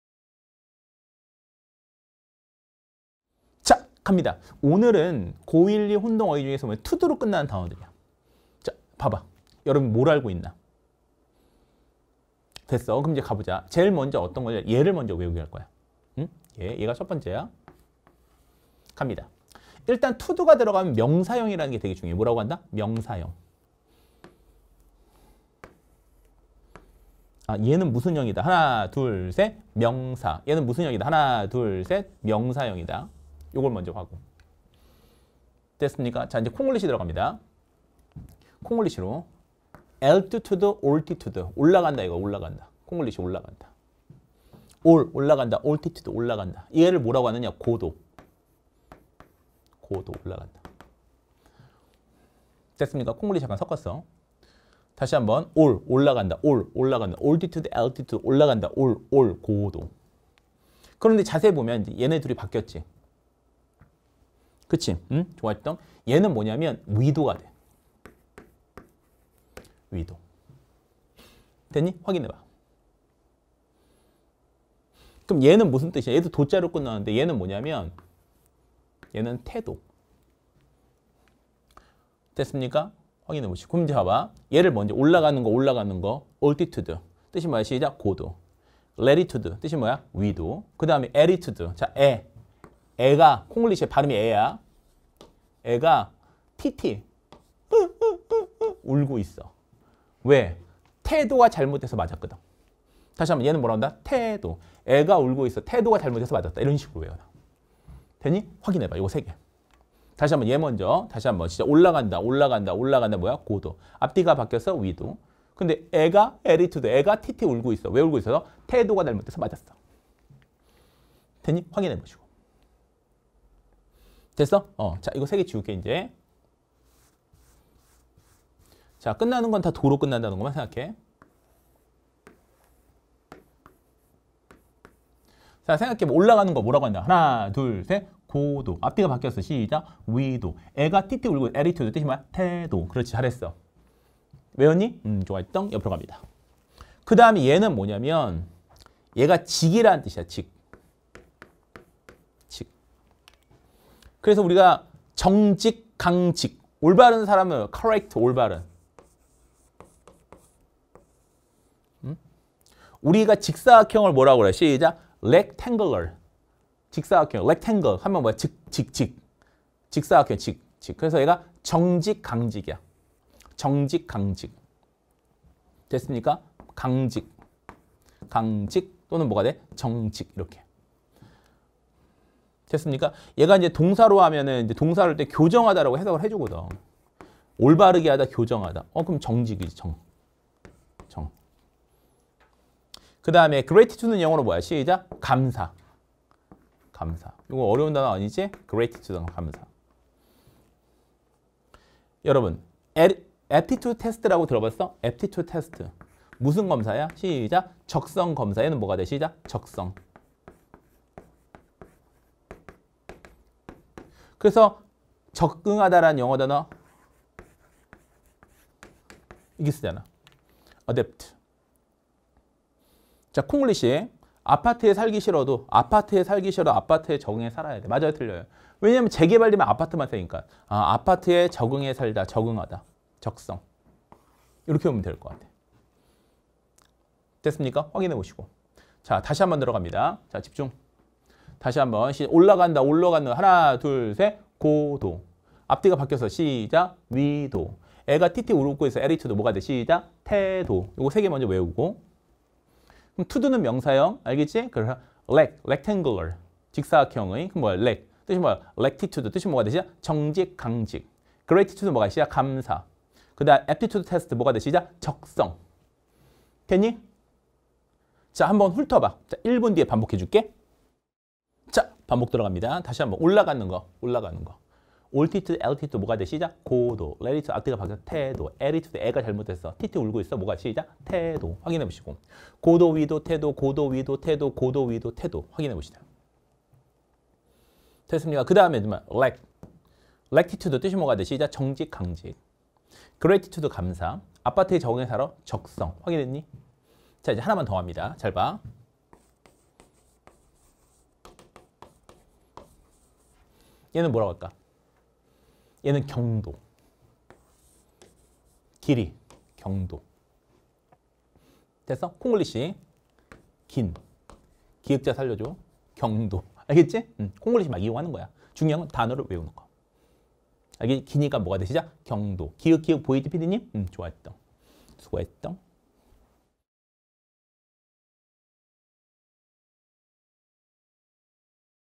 [메가스터디] 영어 단어암기 김동영쌤 - 혼동 어휘 특강 (26)
고 1,2 학생이 알아야 할 혼동 어휘 특강입니다.

김동영쌤과 함께라면 5분 만에 많은 단어를 외울 수 있는 미라클!

지금 미니 특강을 통해 바로 확인하세요!